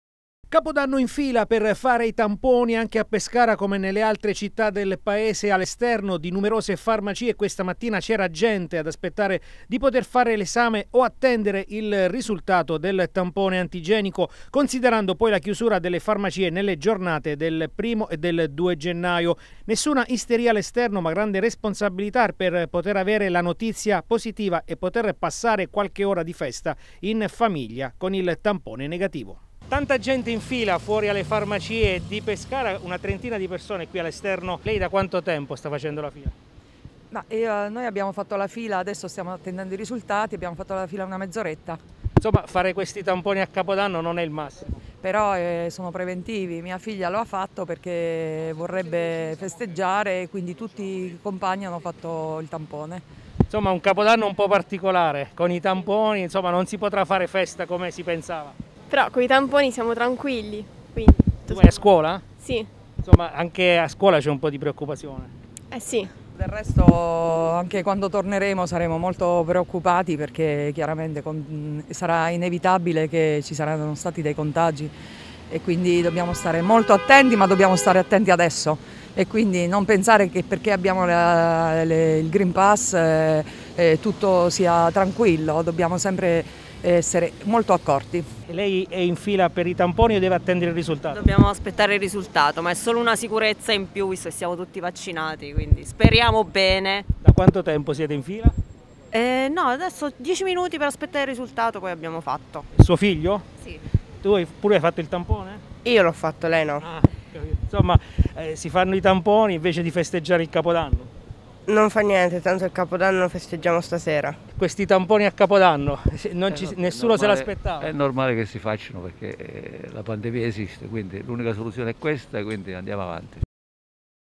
Covid. File fuori dalle farmacie per i tamponi
Lunghe file all’esterno di alcune farmacie in queste ore da parte di persone in attesa di fare il tampone. Noi siamo andati all’esterno di una farmacia di Pescara